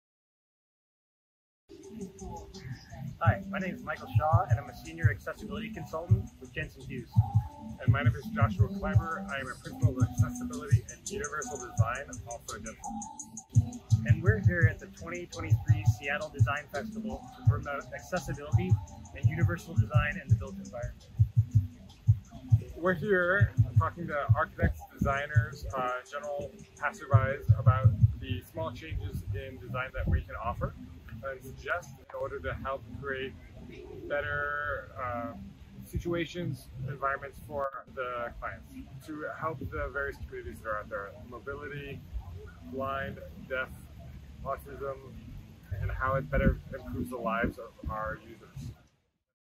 Accessibility + Universal Design at the 2023 Seattle Design Festival | Jensen Hughes
Join Joshua Klyber and Michael Shaw at SDF2023, where they focused on education and promotion of universal design in our communities and built environments.

They worked with participants on live mobility exercises, trivia, thought exercises, and learning about assistive technology and design materials that are universal design-friendly and aesthetically pleasing.

This event was a collaboration between Jensen Hughes, Carli Hoki Designs, Pathway Design and Construction, Washington Assistive Technology Program, the Northwest Universal Design Council and Age Friendly Seattle.

Thank you to Seattle Design Festival and AIA Seattle for hosting this annual community event!